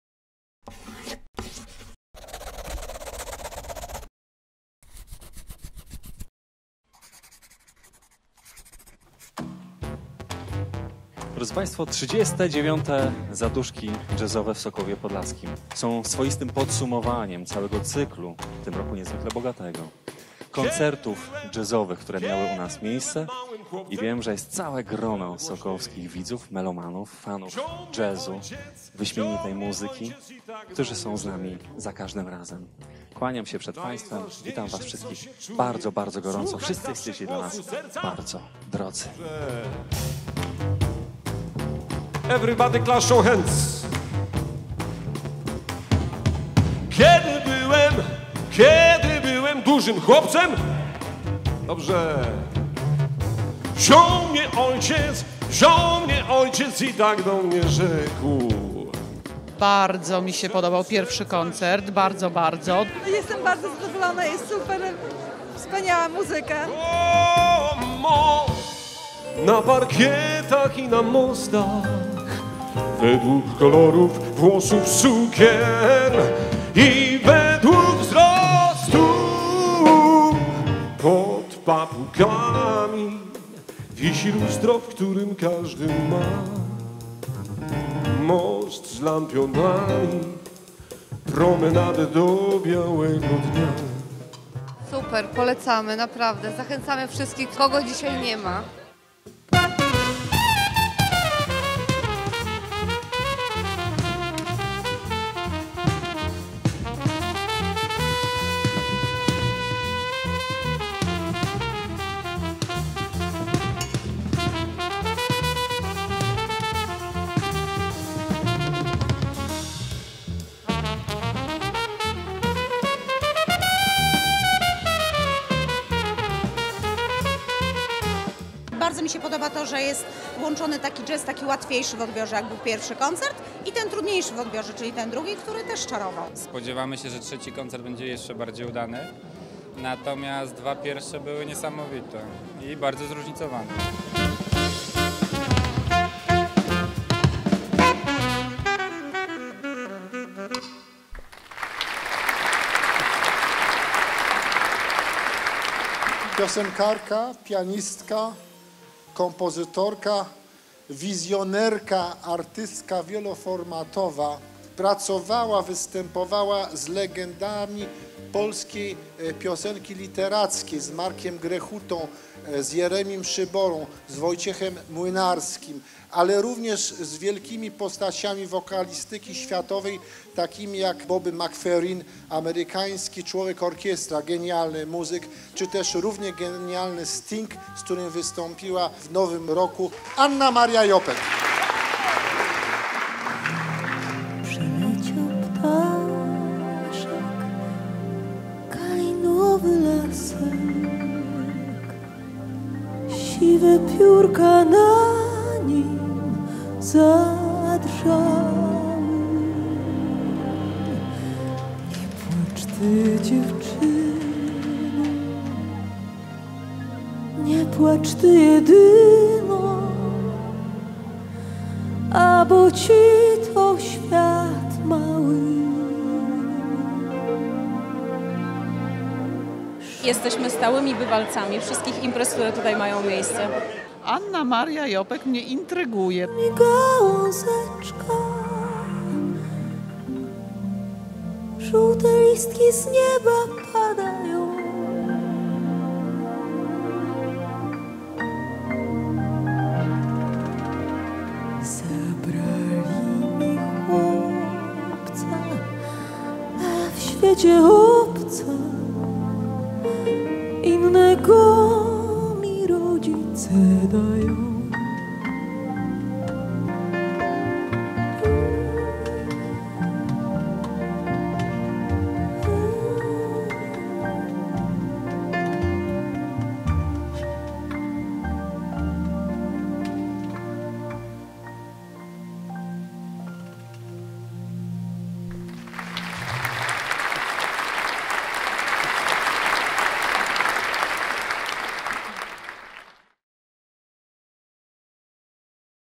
39 ZADUSZKI JAZZOWE
Jesienna tradycja jazzowego koncertowania kontynuowana jest w naszym mieście od 39 lat. Sokołowskie Zaduszki Jazzowe, co roku przyciągają tłumy miłośników tego gatunku muzycznego, a bilety wyprzedawane są z dużym wyprzedzeniem.

Występujący na sokołowskiej scenie artyści, a w tym roku byli to: NSI oraz Janusz Szrom z Zespołem zadbali o nostalgiczny, zaduszkowy nastrój. Gwiazdą tegorocznej edycji była Anna Maria Jopek z Kwarterem. Artystka z charakterystyczną dla siebie lekkością i finezją oczarowała publiczność swoim magicznym głosem.